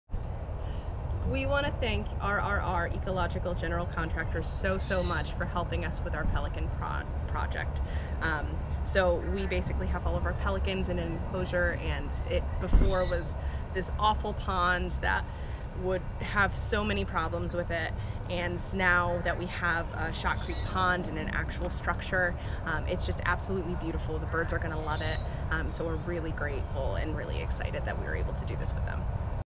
Customer Testimonial - Wild Bird Rescue Park
We are very grateful to all the staff of the Wild Bird Rescue Park, for the help they have offered us and the constant accompaniment in carrying out this great reconditioning and improvement project.

It was a project that we really enjoyed doing and we are especially proud of its ecological and environmental contribution.

Always grateful to our customers.

If you can imagine it, we can build it.

RRR General Contractor.